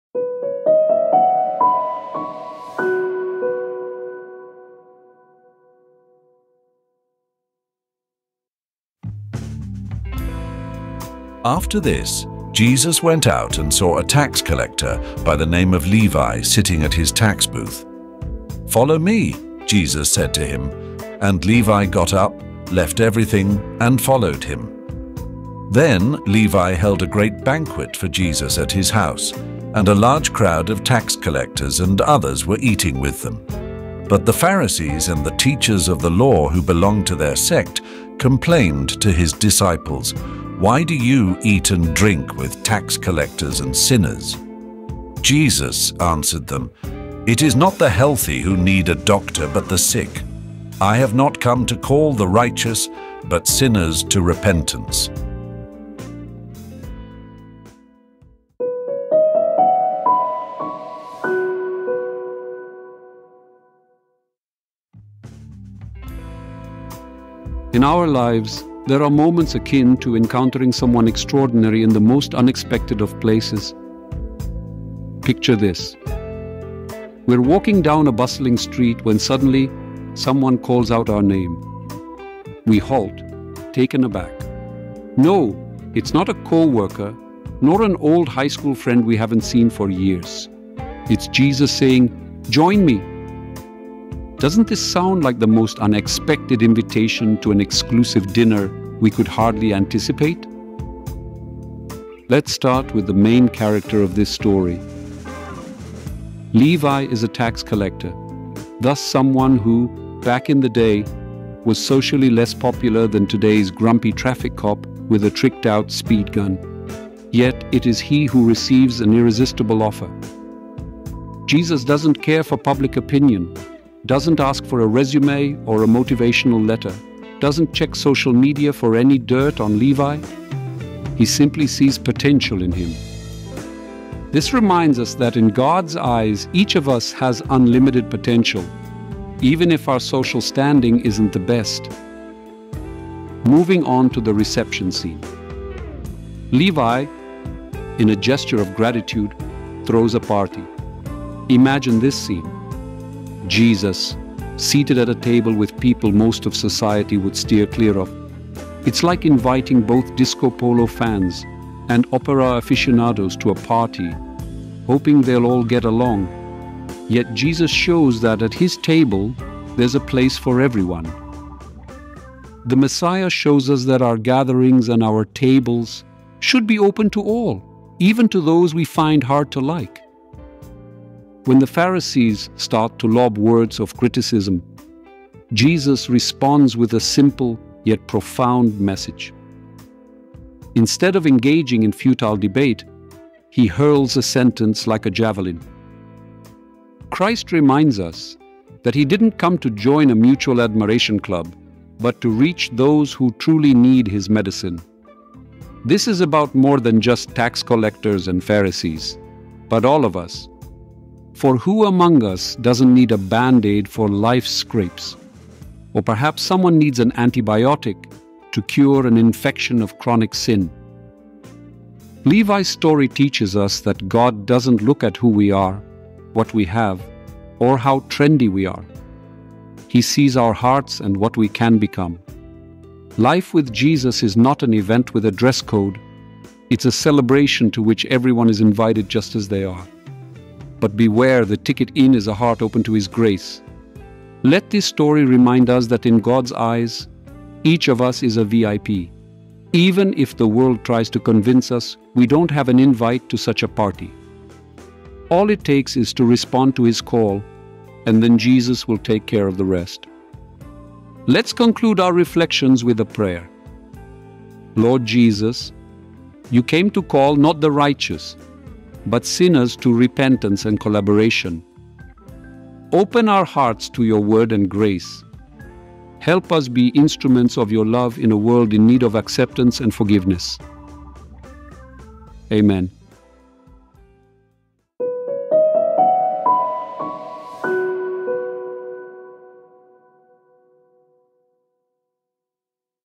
DINNER WITH THE ENEMY - UNDAILY Episode 64
After this, Jesus went out and saw a tax collector by the name of Levi sitting at his tax booth. “Follow me,”Jesus said to him, and Levi got up, left everything and followed him. Then Levi held a great banquet for Jesus at his house, and a large crowd of tax collectors and others were eating with them. But the Pharisees and the teachers of the law who belonged to their sect complained to his disciples, “Why do you eat and drink with tax collectors and sinners?” Jesus answered them, “It is not the healthy who need a doctor, but the sick. I have not come to call the righteous, but sinners to repentance.” LUKE 5, 27-32 (NIV)